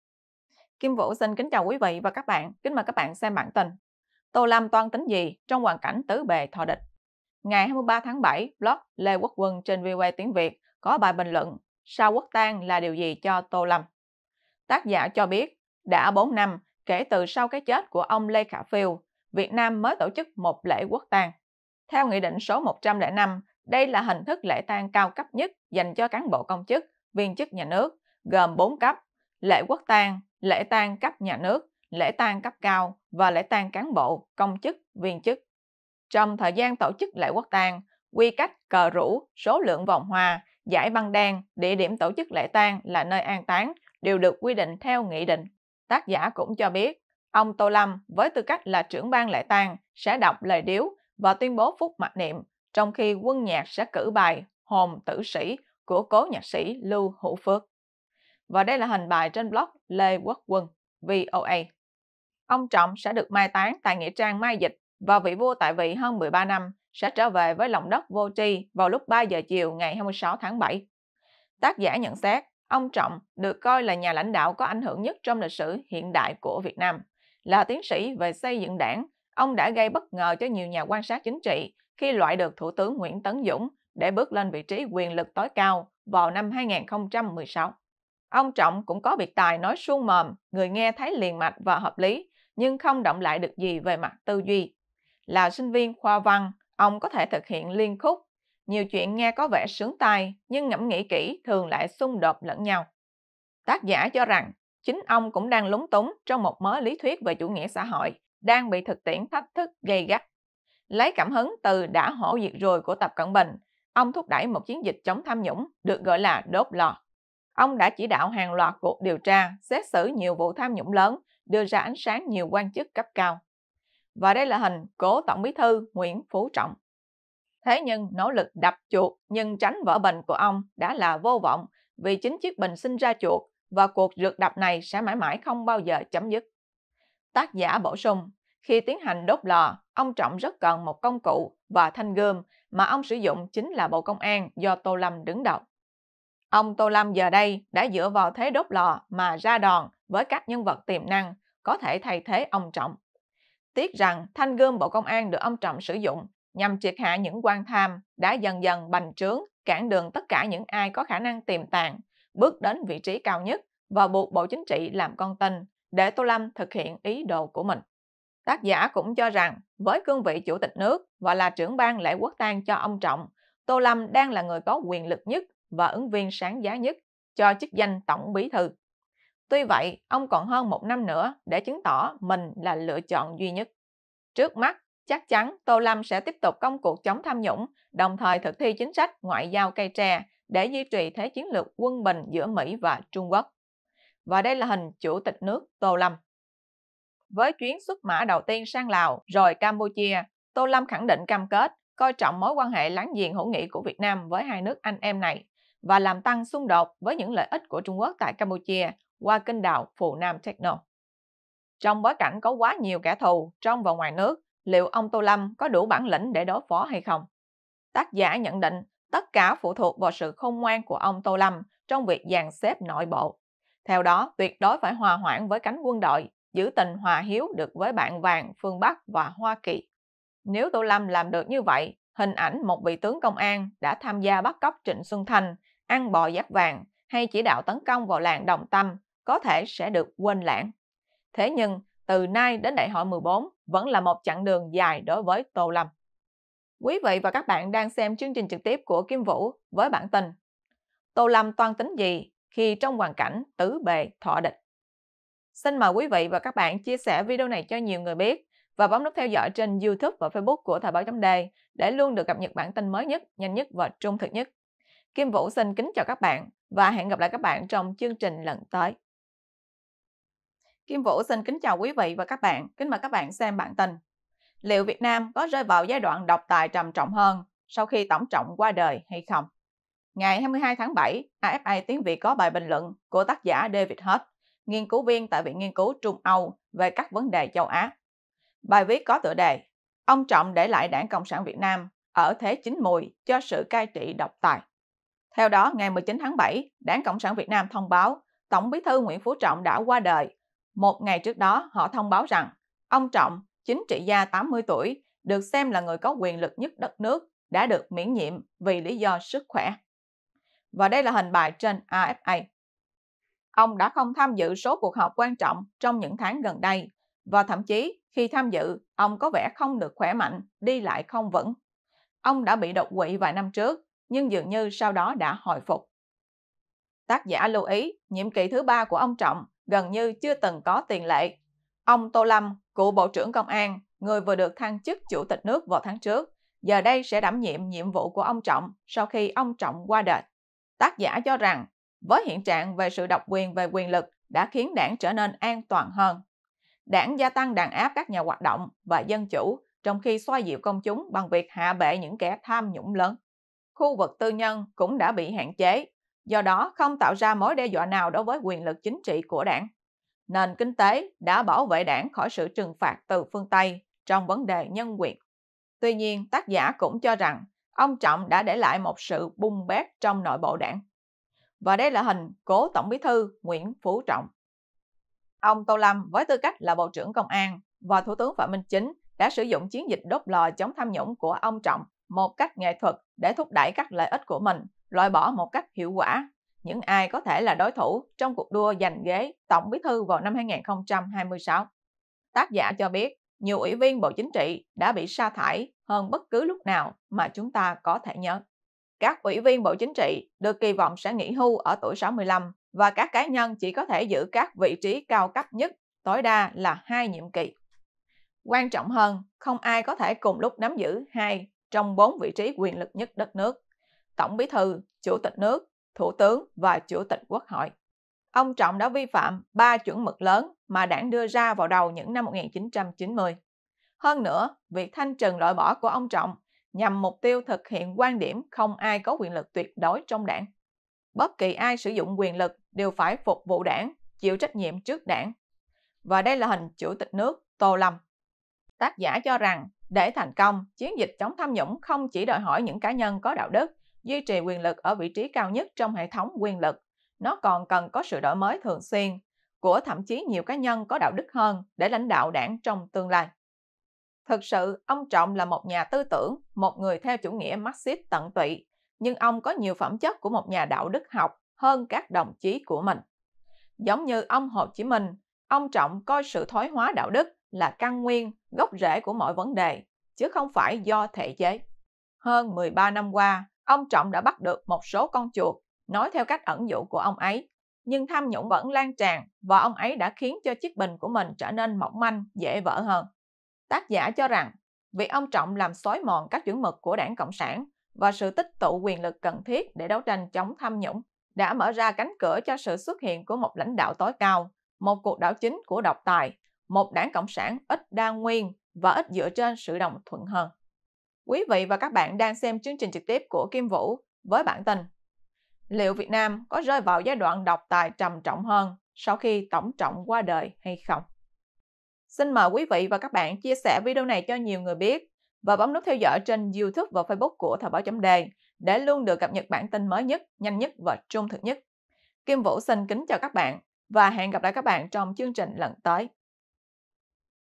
Tô Đại toan tính gì khi trong hoàn cảnh “tứ bề thọ địch”
Với cương vị Chủ tịch nước và là Trưởng ban Lễ Quốc tang cho ông Trọng, Tô Lâm đang là người có quyền lực nhất, và ứng viên sáng giá nhất cho chức danh Tổng Bí thư. Tuy vậy, ông sẽ còn hơn 1 năm nữa, để chứng tỏ mình là lựa chọn duy nhất.
Bạn cài Ultrasurf để vượt tường hoặc xem qua link vượt tường